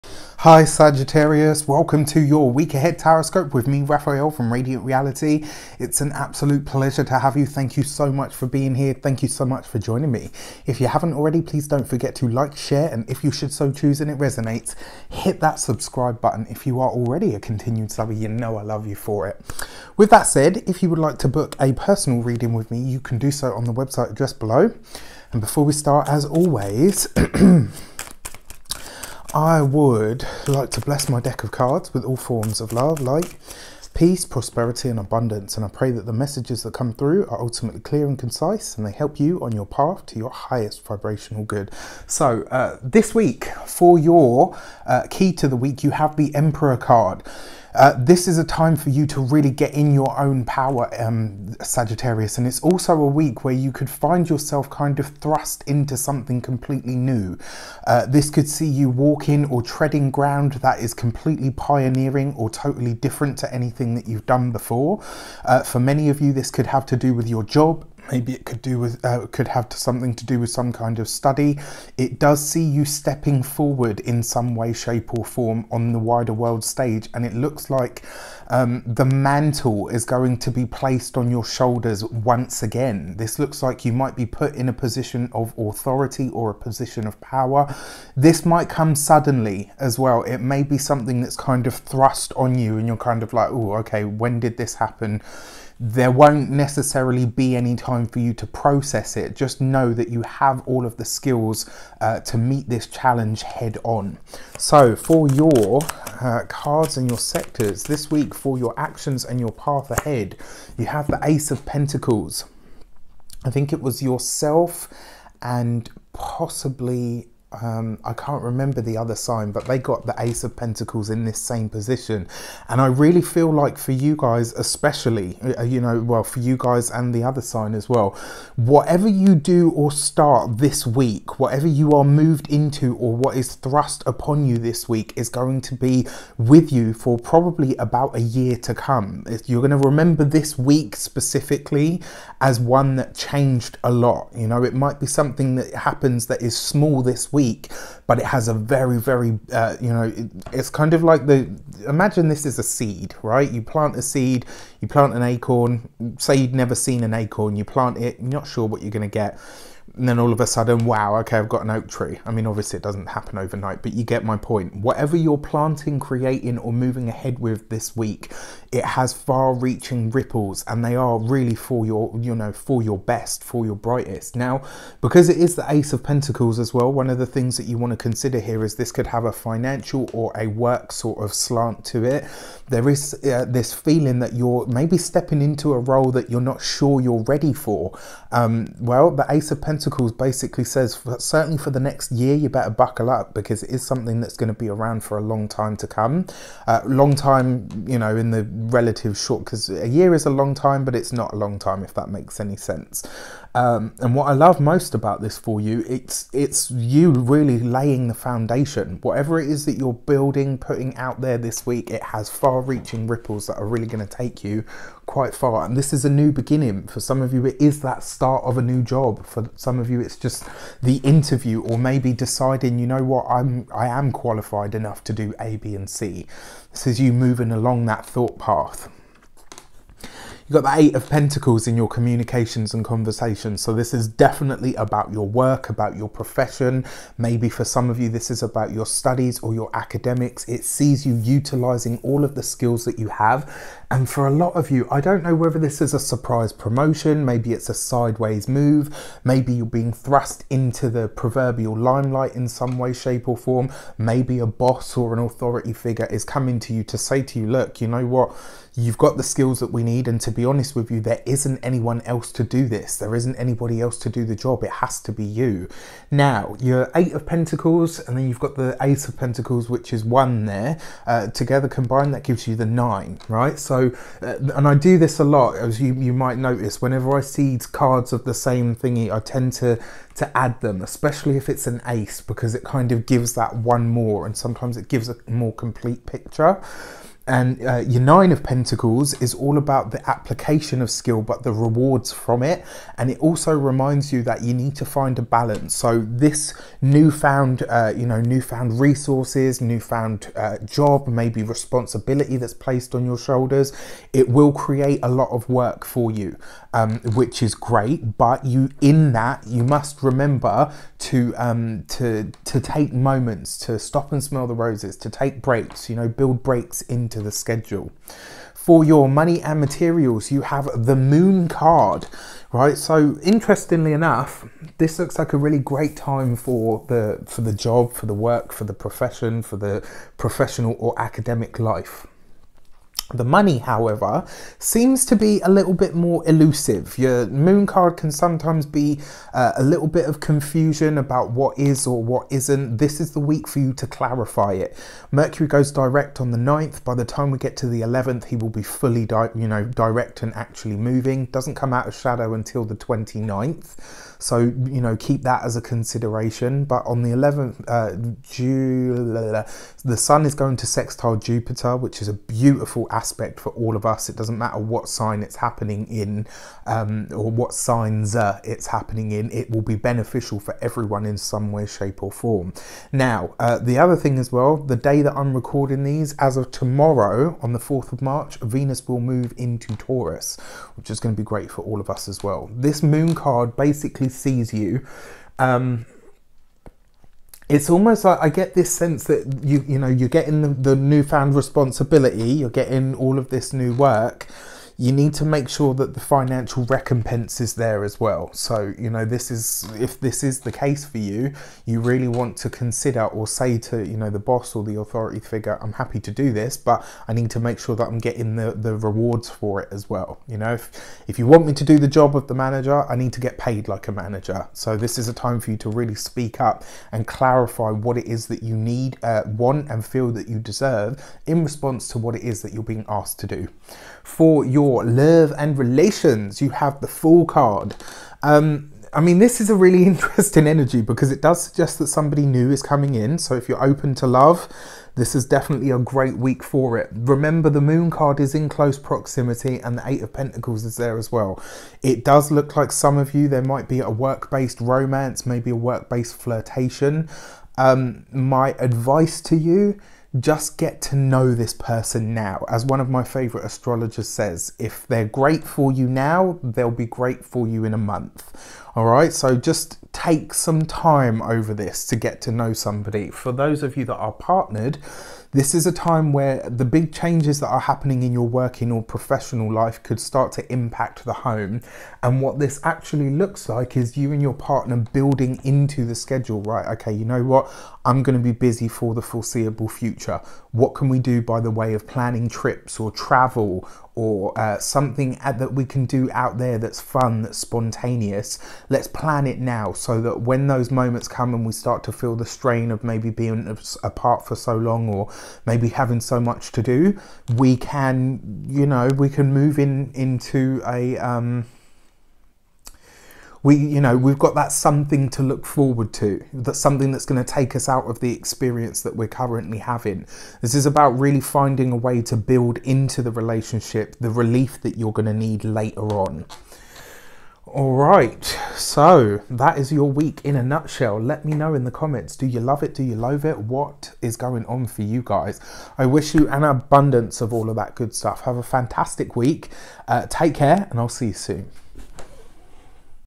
Sagittarius Weekly Tarot - **Work LIFE gets a BIG BOOST !** - 9th-15th March 2020
The Sagittarius Weekly Tarot Reading is an insight into what the week ahead is like in its energy, outlook and general trends and themes. This will focus on love, money, Actions and Communication.

Join Reydiant Reality!